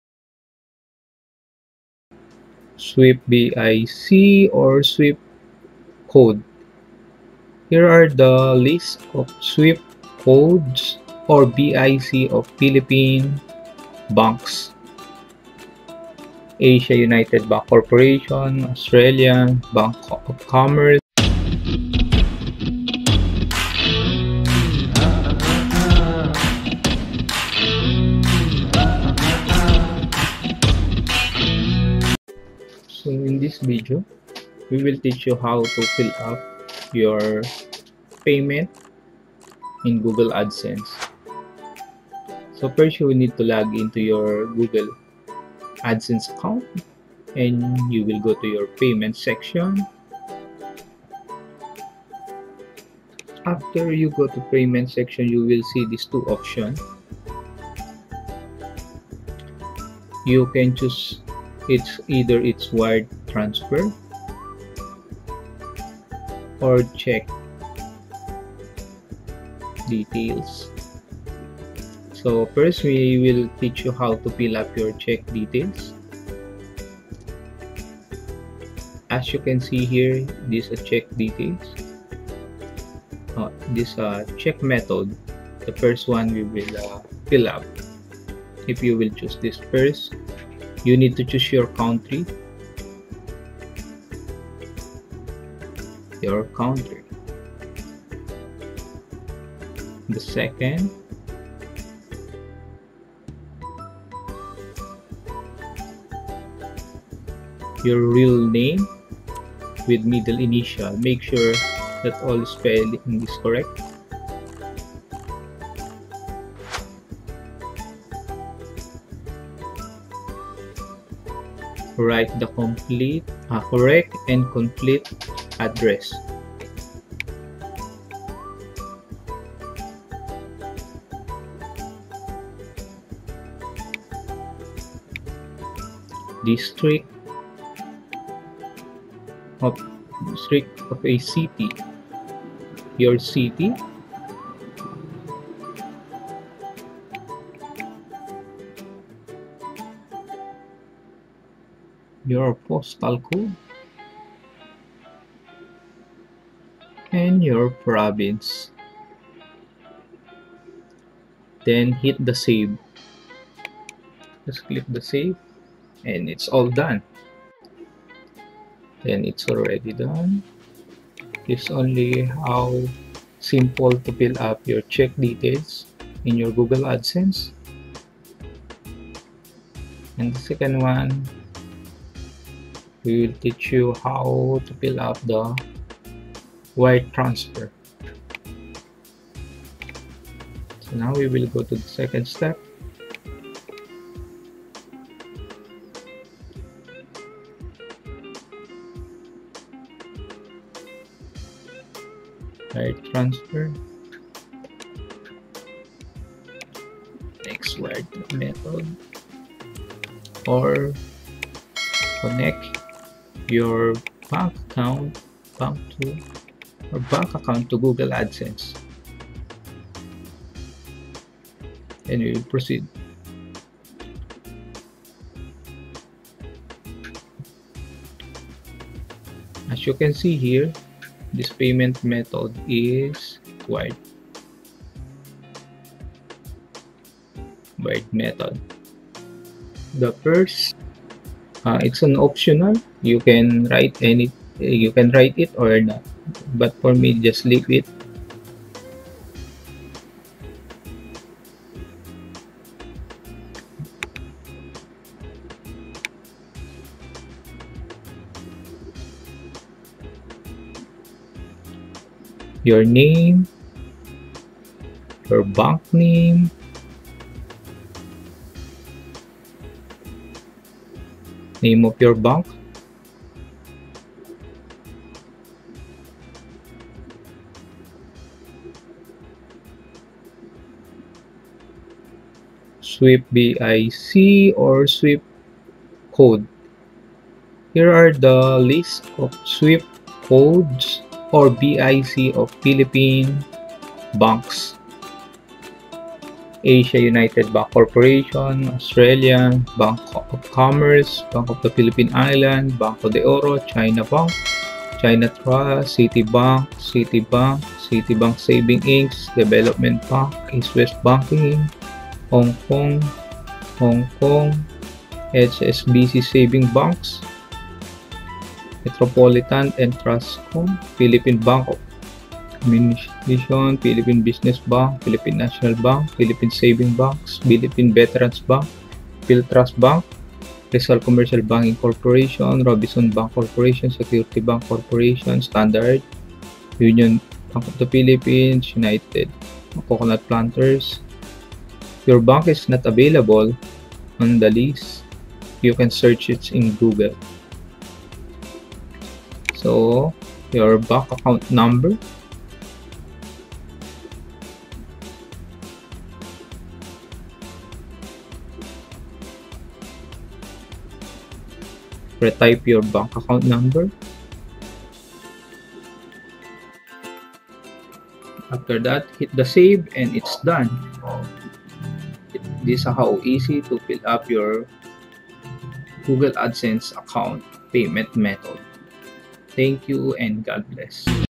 How to Setup your AdSense Payment Account
Here is the step-by-step tutorial on setting your google payment method in two options wired transfer or check method.

Here you will learn to set up, and Awith BIC or CODe is also available in video Swift BIC of a bank in the Philippines